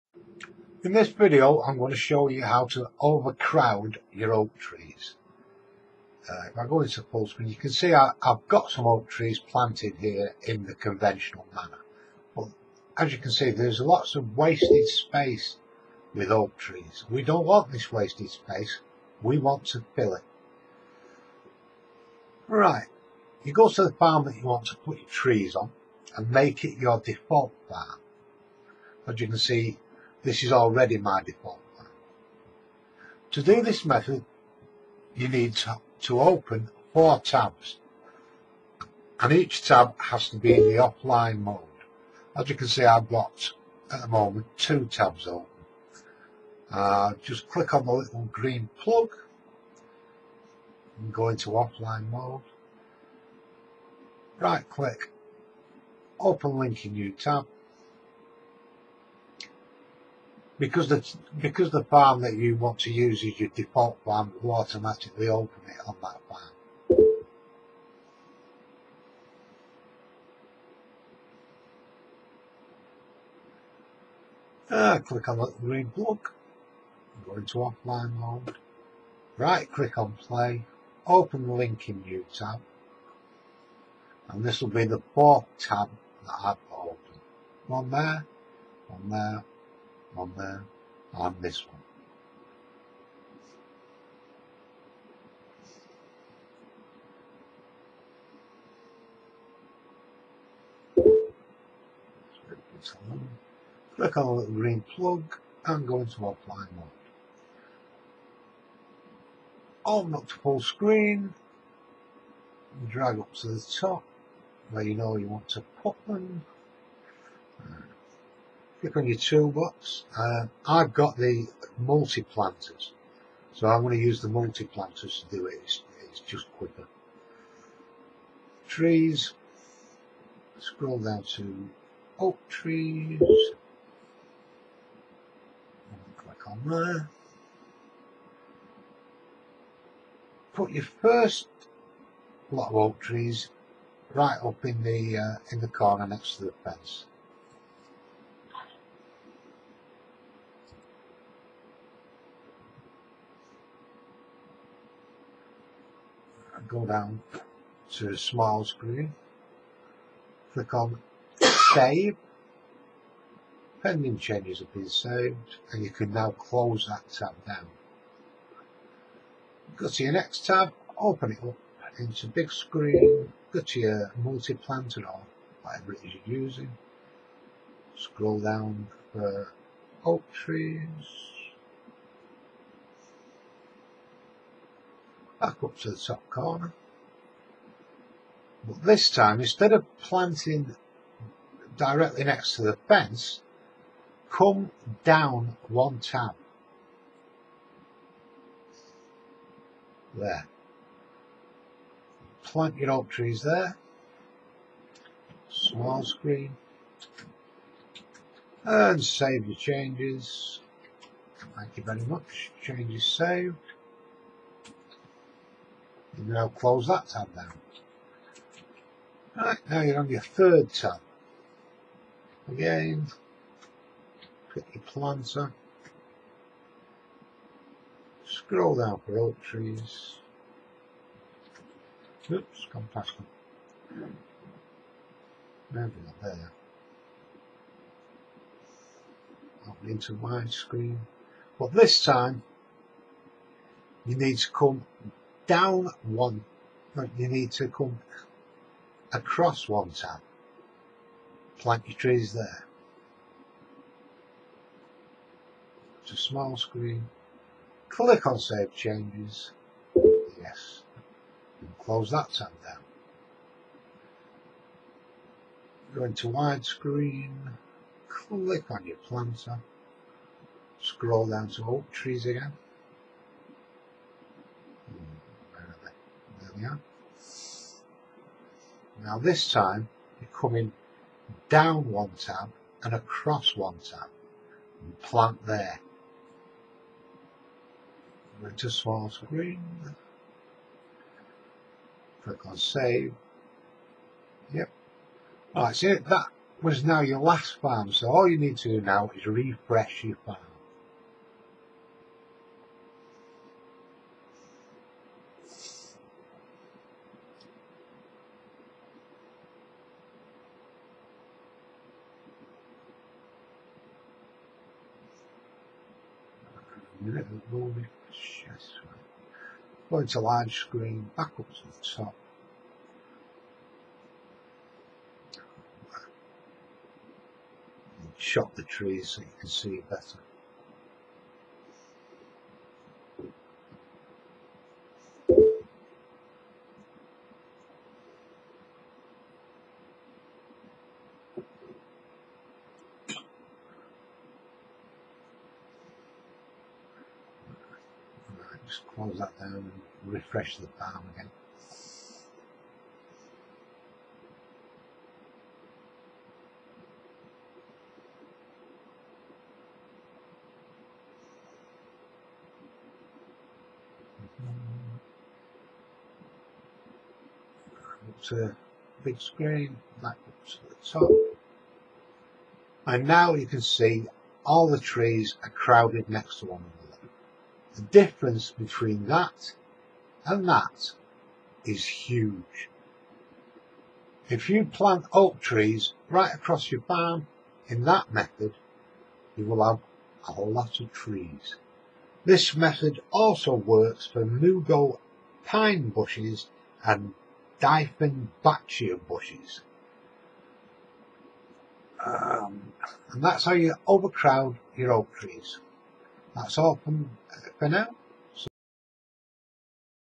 overcrowding oak trees
how to overcrowd your oak trees,mugo pine bush and dieffenbachia bush